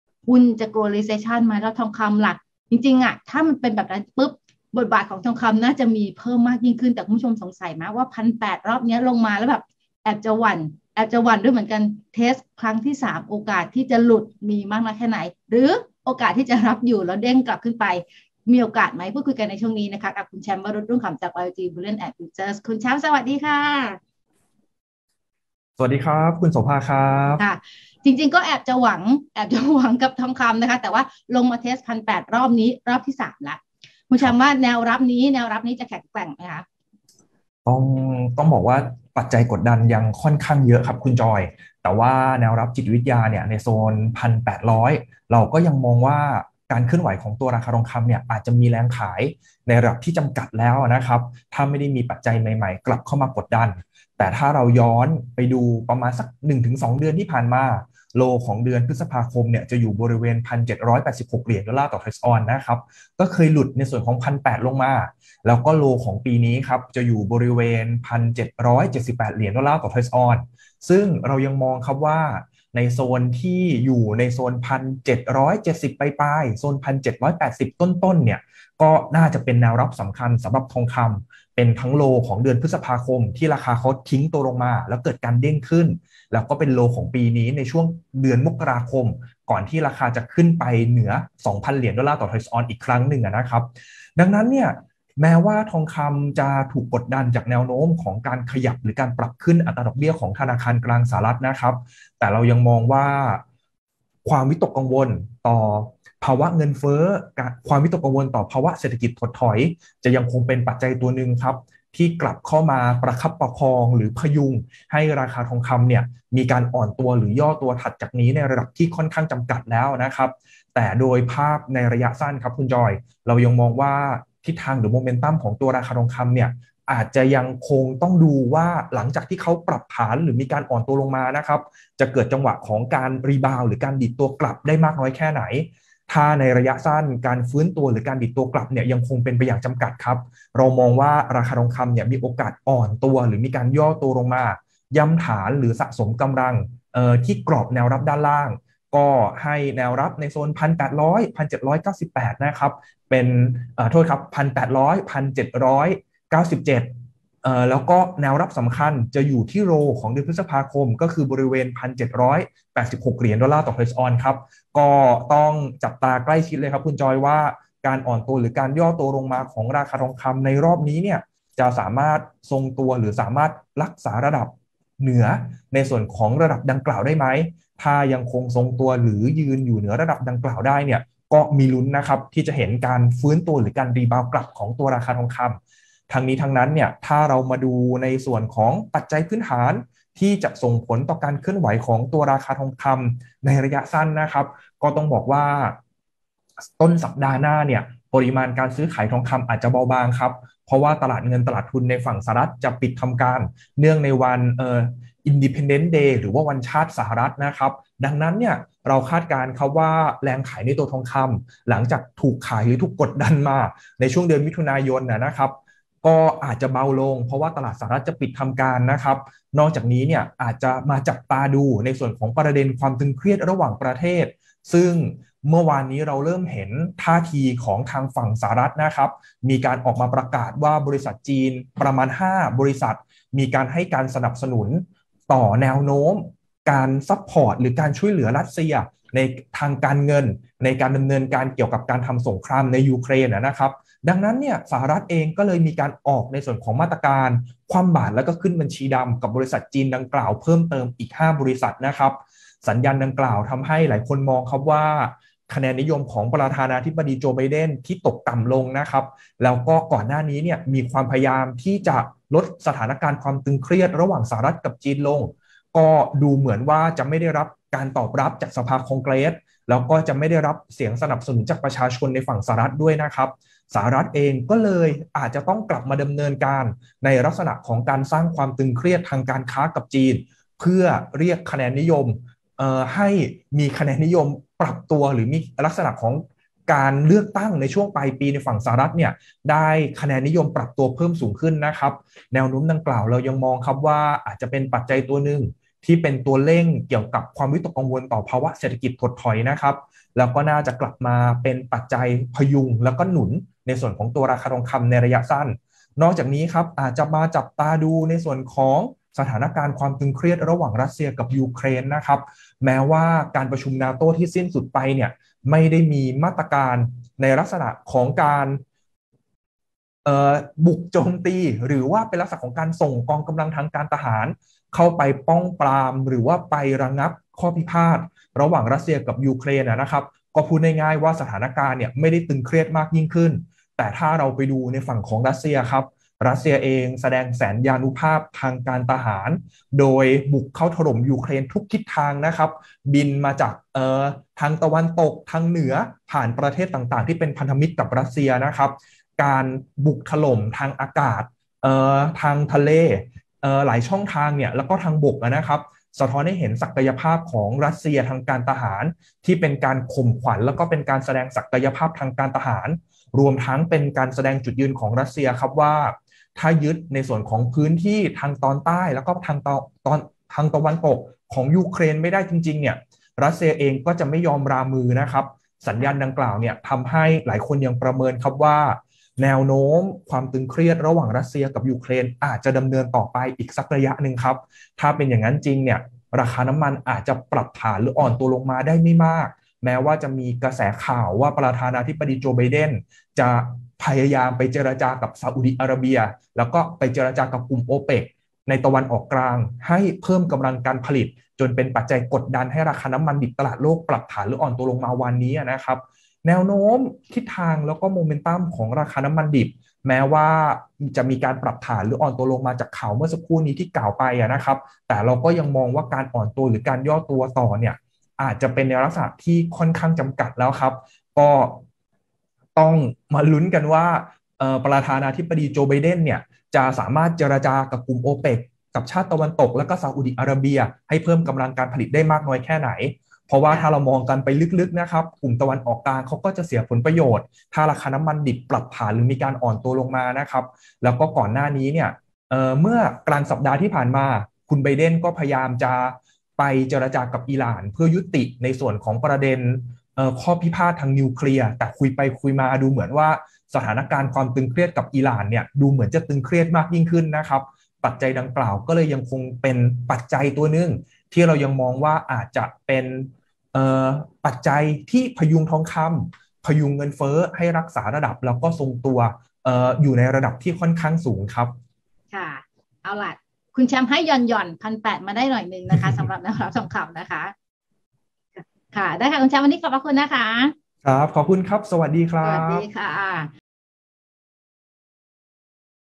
จับตาราคาทองคำ 1,800 $ รับอยู่หรือไม่ I TNN WEALTH 01-07-65
"จับตาราคาทองคำ 1,800 $ รับอยู่หรือไม่" กับคุณวรุต รุ่งขำ ผู้อำนวยการฝ่ายวิเคราะห์ บริษัท วายแอลจี บูลเลี่ยน แอนด์ ฟิวเจอร์ส
ติดตามข่าวหุ้นและการลงทุนทางไลน์
ติดตาม TNN Wealth ผ่านช่องทางต่าง ๆ ได้ที่
หรือดูรายการ Live ได้ทาง